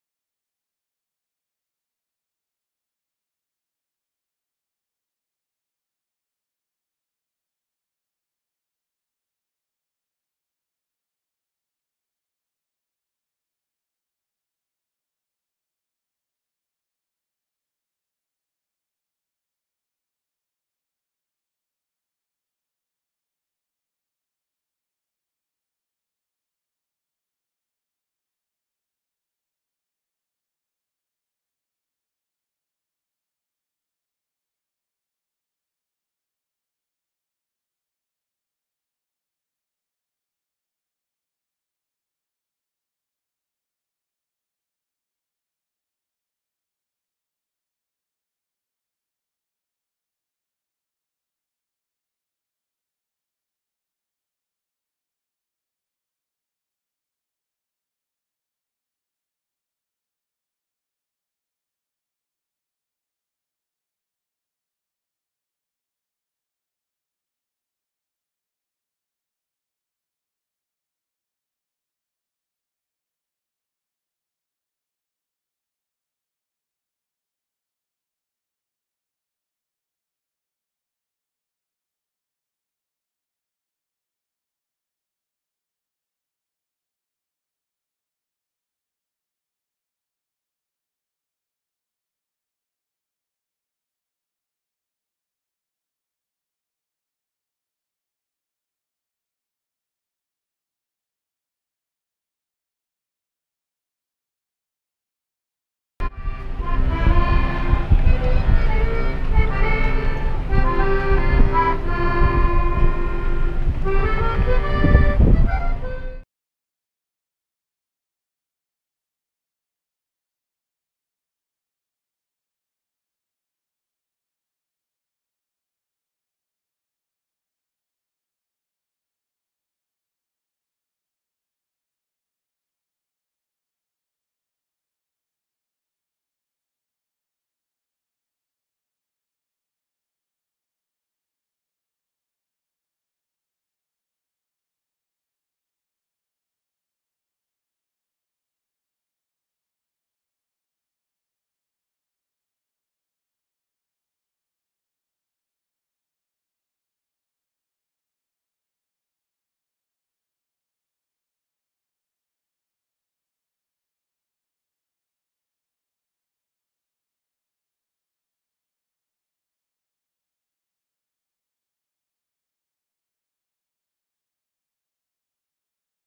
Old San Juan Streets
Paseo por las calles del viejo San Juan Enero 1, 2017